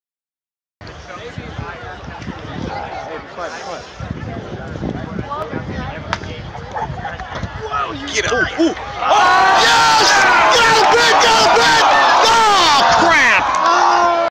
Callie Hicks 12 ft attempt, dad loses it
4/24/15 Free State Barrier Breakers meet. Callie Hicks skims the bar in her 12 ft attempt (would have been a new PR), it hangs two seconds then falls. It would have kept her in the running for 1st place. Dad sort of lost it. So sue me.